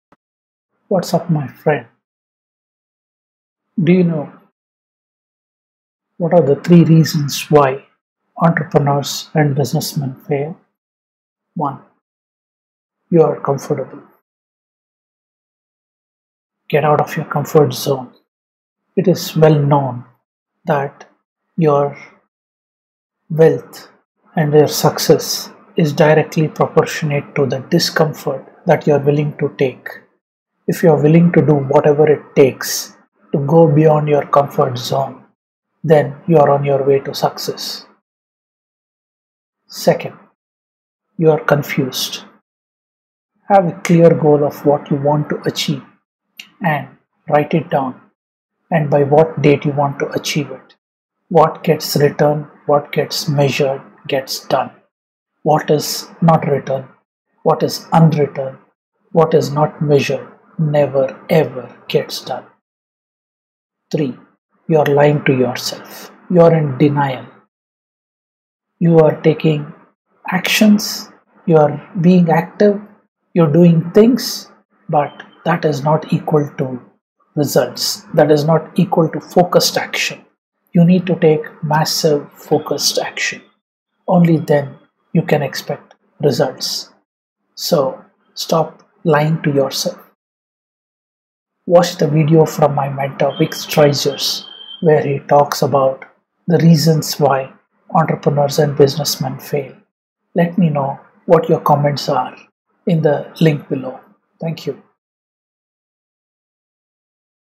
Why Do Entrepreneurs Fail
. An Introductory video as to why Online Entrepreneurs (DigiPreneurs) fail. The video looks at the 3 primary reasons for failure and provides a intro to a detailed video which dives deep to unearth the reasons.